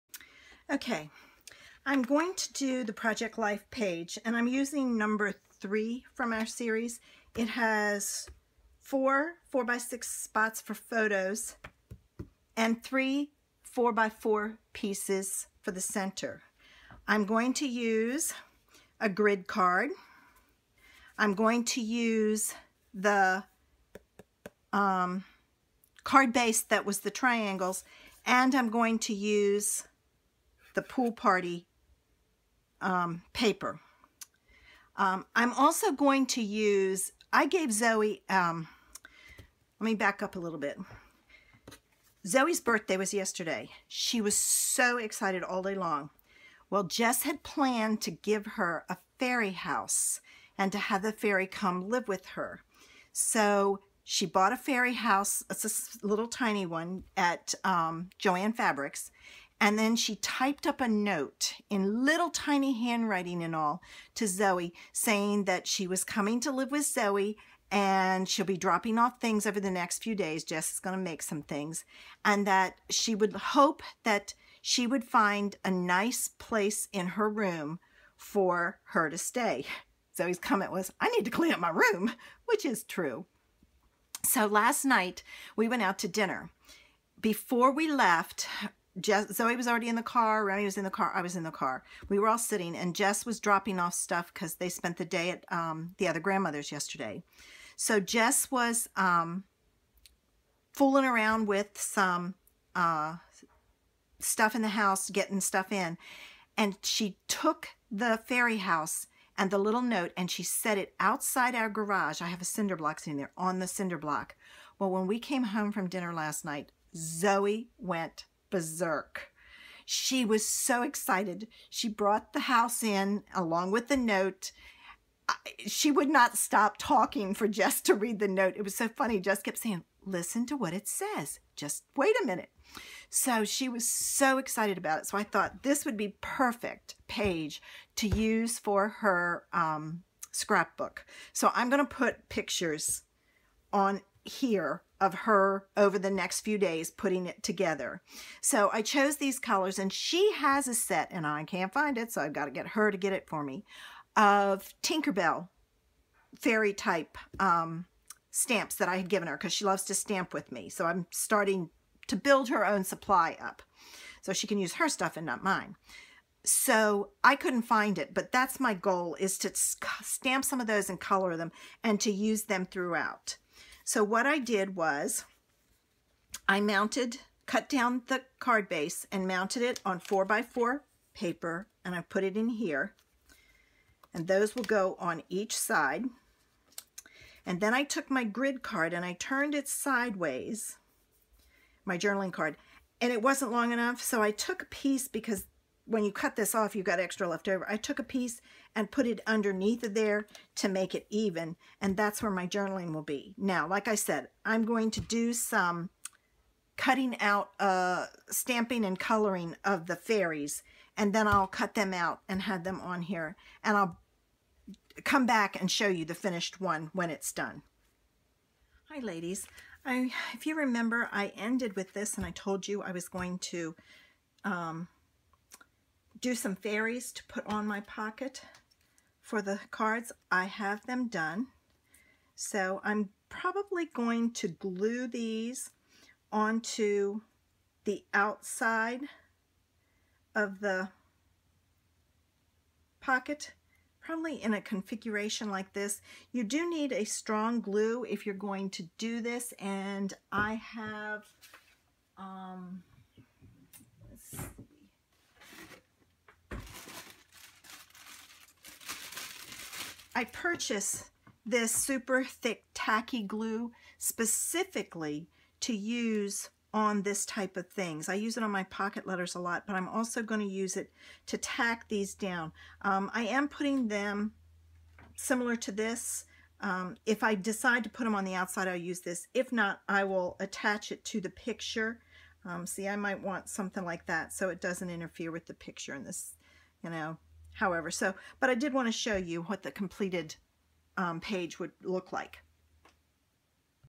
FEBRUARY PAPER PUMPKIN PROJECT LIFE PAGE
I used the February Paper Pumpkin to make this Project Life page.  I will be putting pictures of Zoie's Fairy House and other fairy things in it.